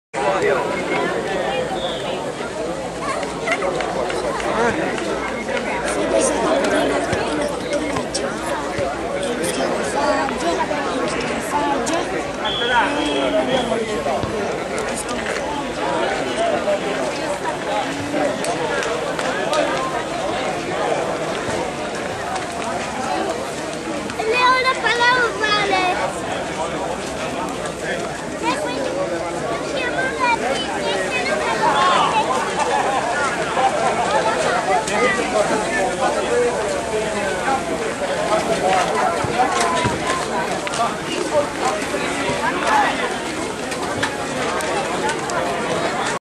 EGOR 2012 Trieste, Prosecco
A Prosecco, sul carso triestino dal 14 al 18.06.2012 si è tenuta la undicesima edizione di EGOR (European Golden Oldies) Festival del rugby, con la partecipazione di 1.500 atleti. Qua chi si riposa e chi gioca